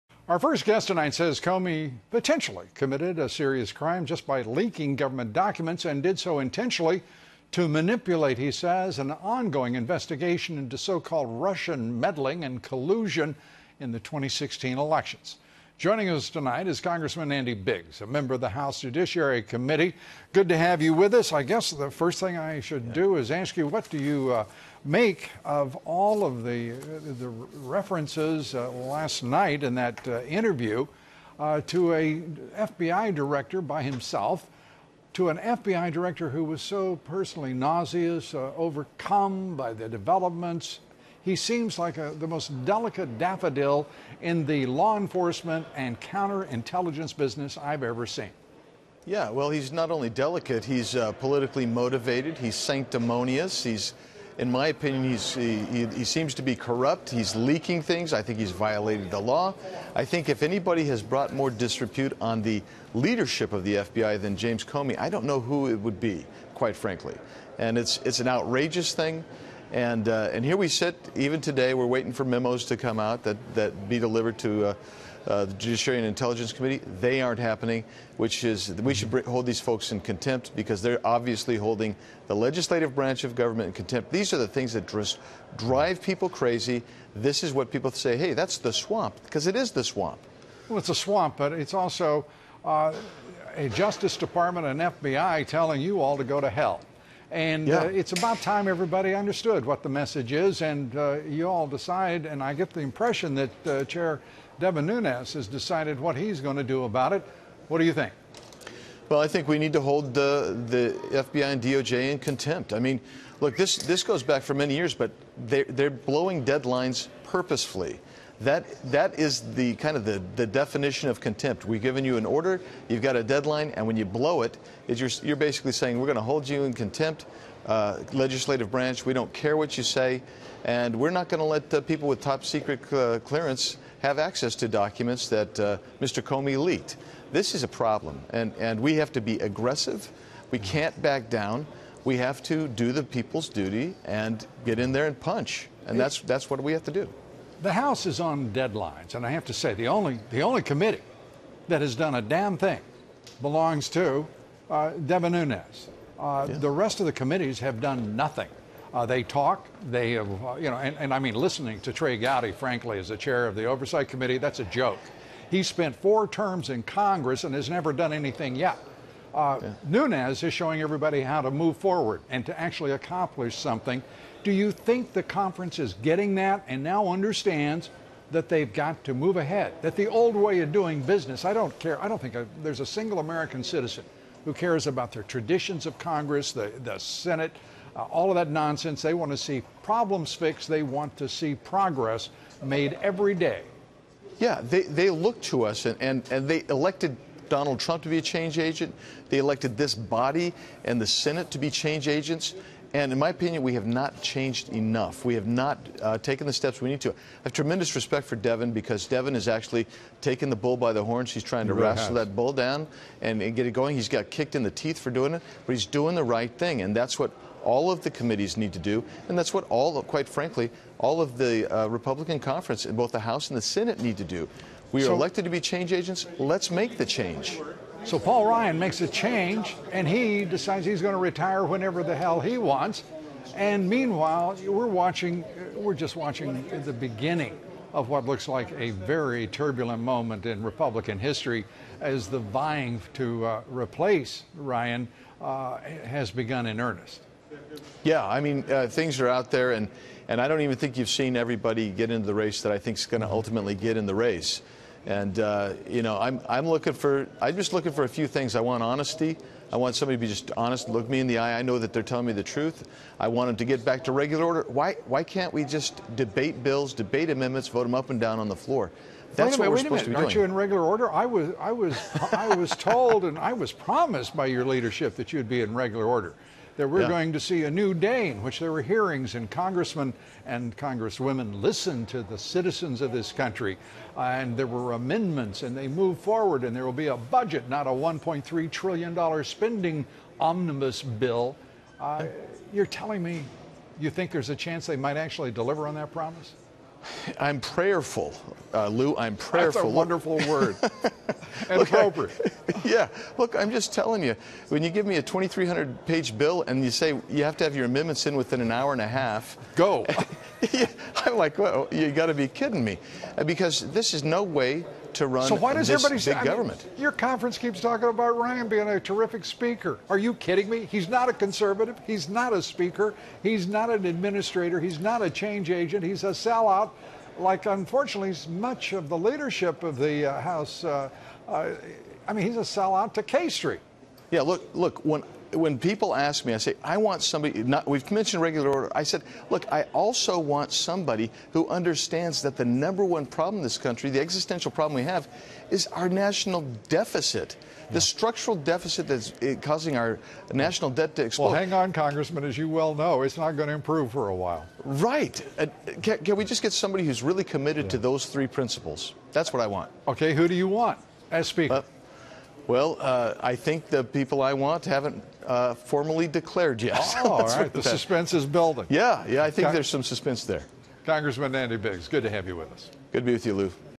FBI, DOJ need to be held in contempt: Rep. Biggs
Rep. Andy Biggs (R-Ariz.) discusses former FBI Director James Comey’s interview on ABC and why the DOJ, FBI need to be held in contempt of court.